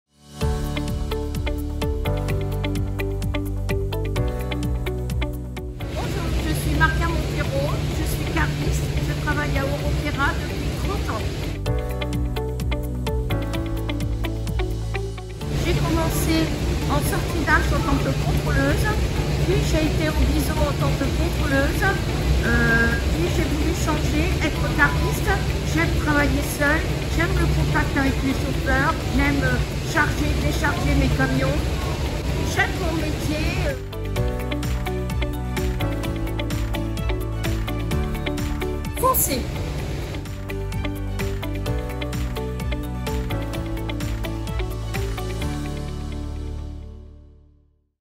Maria - Portrait de femme EuroKera
Maria travaille chez EuroKera depuis 30 ans. De contrôleuse qualité, elle est devenue cariste!
Chez EuroKera, il n’y a pas de métiers inaccessibles aux femmes.